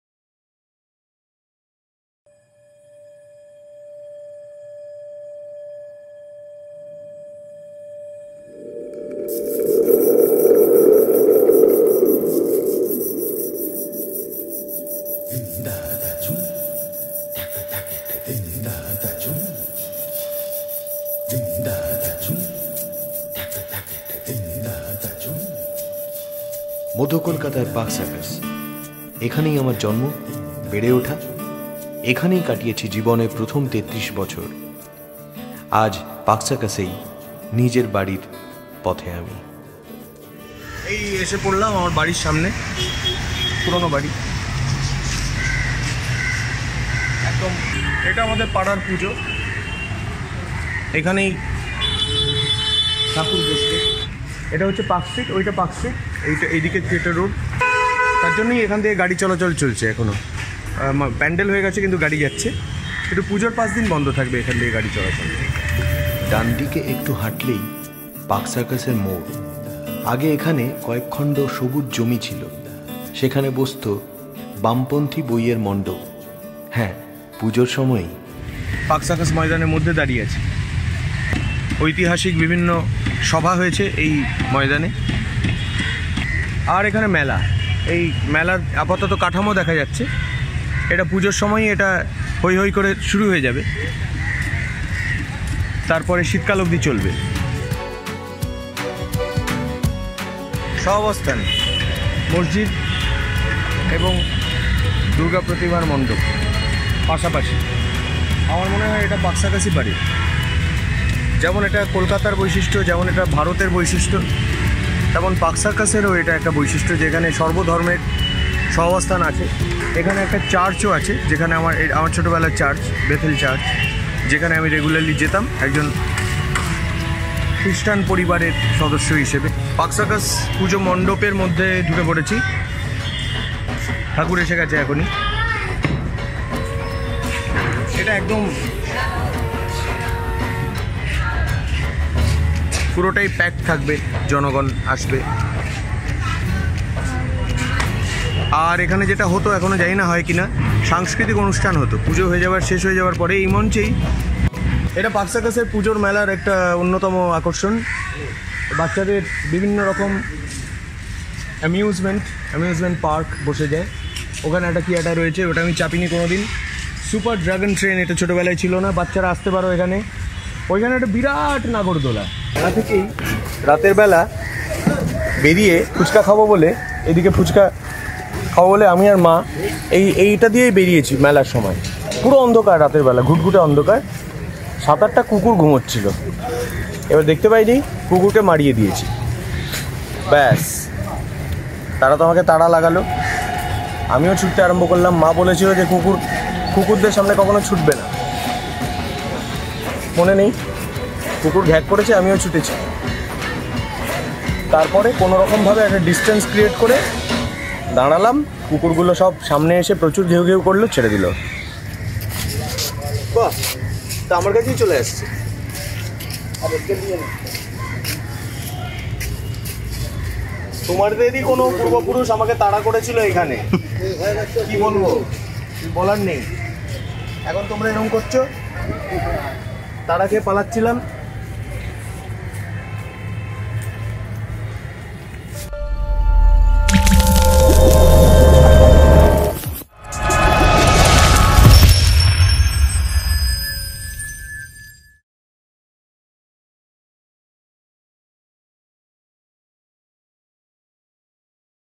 Rupam Islam Unplugged: Home for 33 Years | Durga Puja 2019
'কলকাতা: রূপমের কথকতা' - তৃতীয় পর্ব!

আমার জীবনের প্রথম ৩৩ বছরের বাড়ি, আমার পাড়ার পুজো ঘুরে আসা। স্মৃতিতে জড়িয়ে আছে একদল কুকুরের তাড়া খাওয়ার গল্পও!